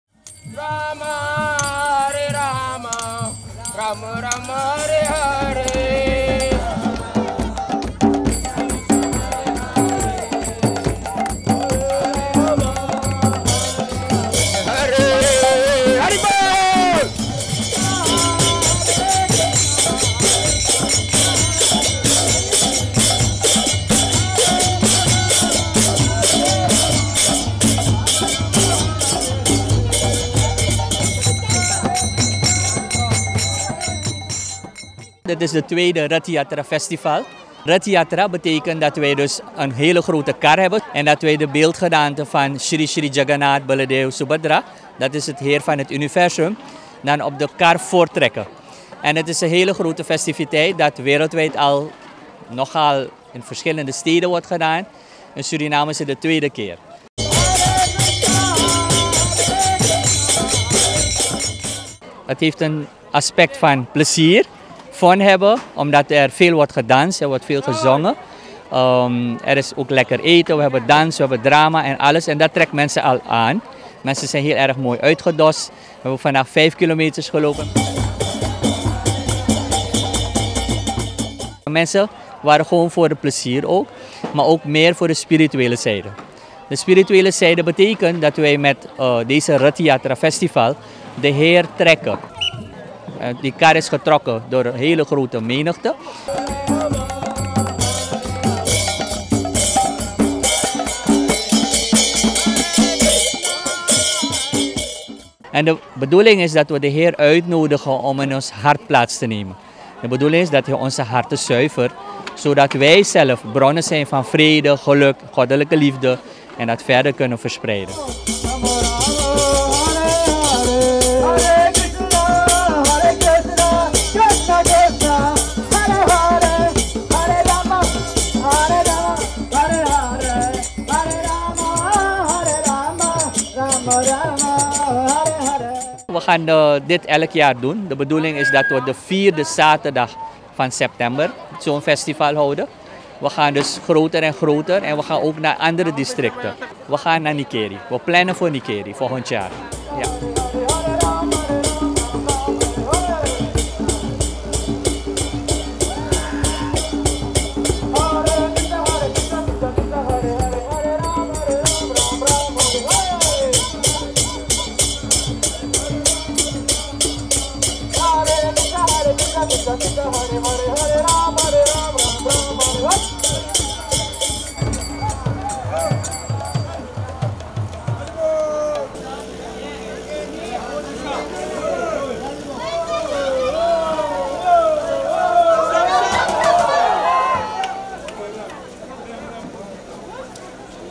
Kar-festival wordt elk jaar grootser van opzet
De tweede Ratha Yatra (Kar-festival) is achter de rug.  De processie is gestart bij het Kwatta Sport Complex en is geeindigd bij het ISKCON gebouw (The International Society for Krishna Consciousness) meer bekend als de Hare Krishna Tempel, 5 kilometers verder.